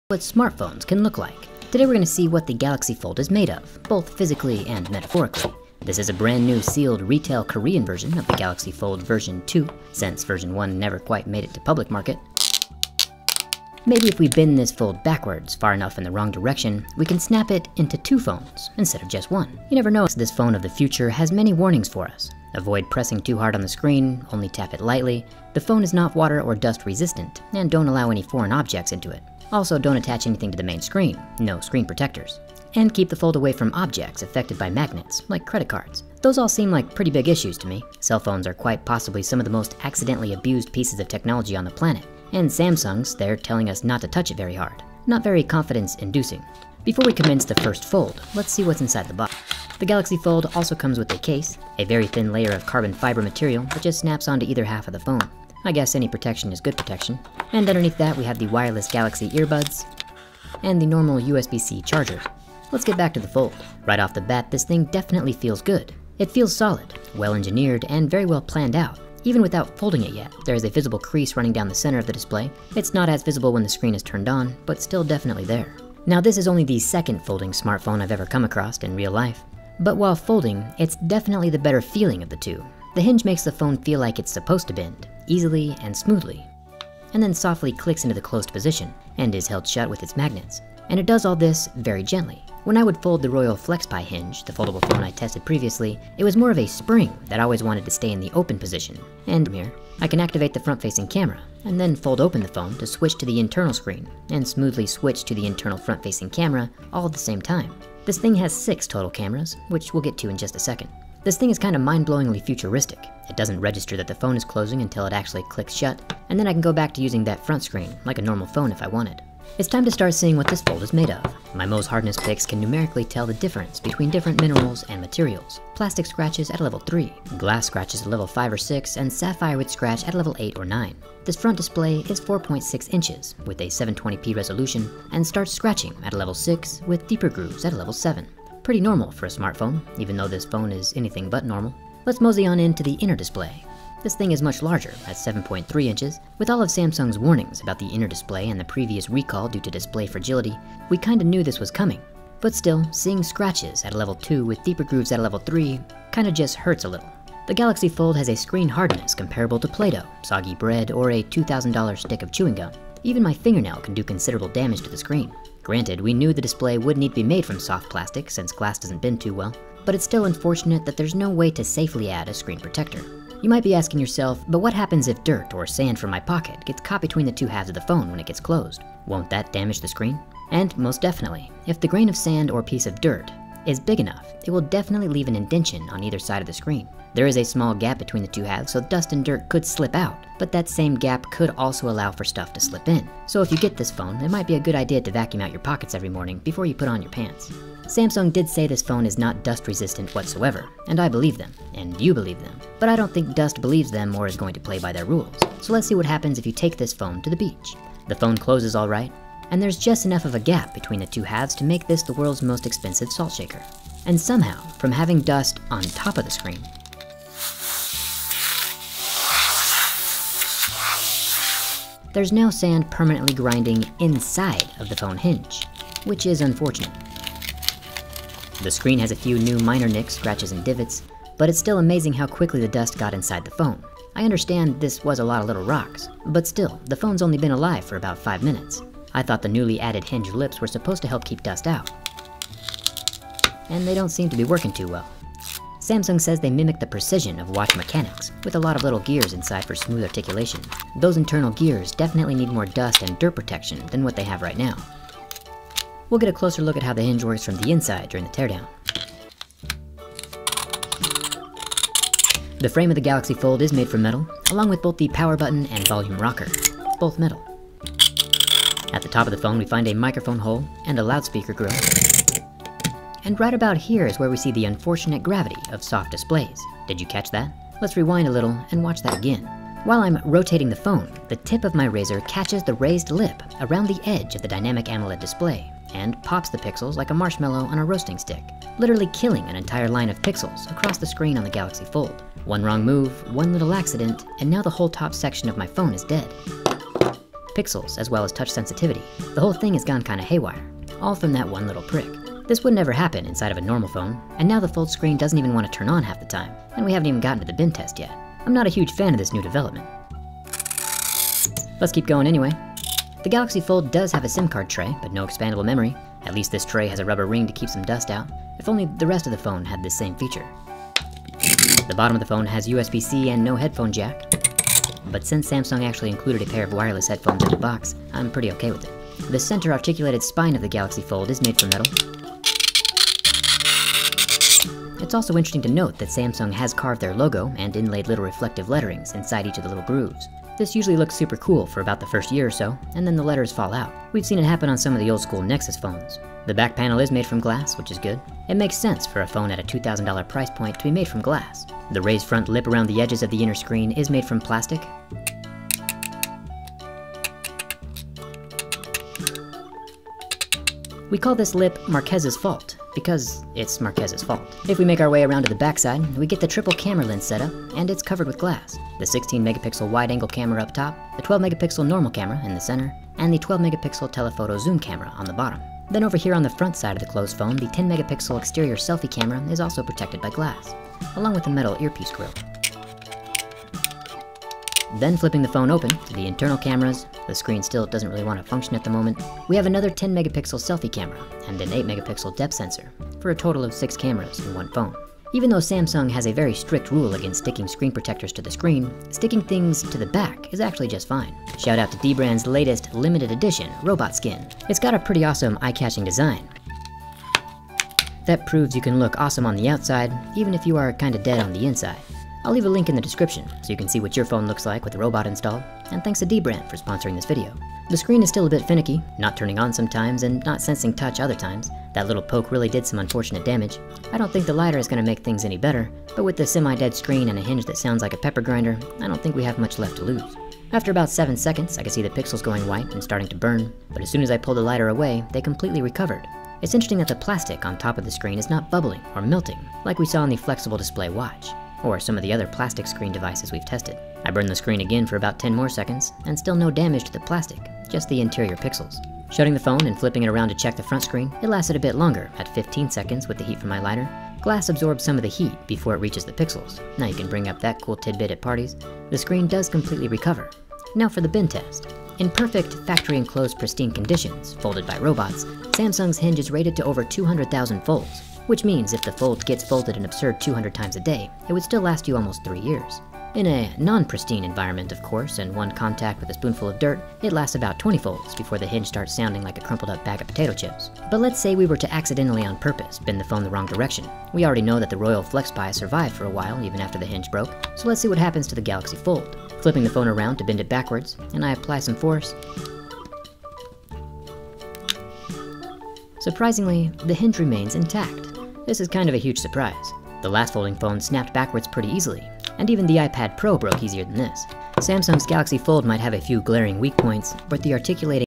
Samsung Galaxy Fold Durability Test! Is it STILL fragile!
Samsung Galaxy Fold Review: Early Verdict
Folding the screen away feels solid, sturdy. Samsung is confident the Infinity Flex display will last for 200,000 folds, and that definitely feels achievable. ... And you can feel the crease as you run your fingertip across the display.